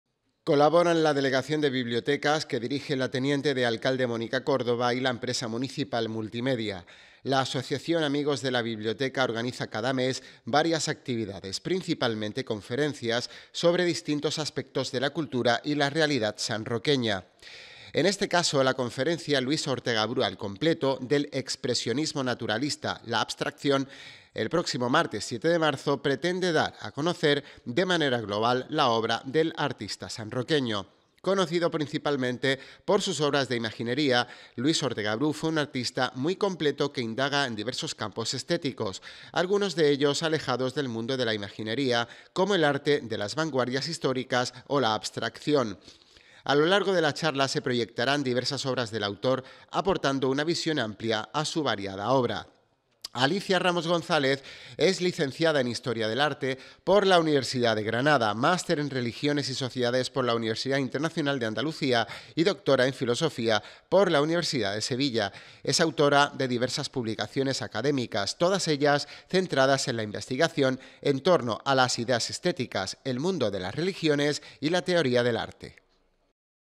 NOTICIA PREVIA CONFERENCIA AMIGOS DE LA BIBLIOTECA 3 MARZO 2023
La Asociación Amigos de la Biblioteca de San Roque organiza para el próximo martes, 7 de marzo, la conferencia “Luis Ortega Brú al completo, del expresionismo naturalista a la abstracción”, a cargo de Alicia Ramos, licenciada en Historia del Arte. Esta actividad se desarrollará en el salón de actos del Edificio Diego Salinas a partir de las 18.30 horas.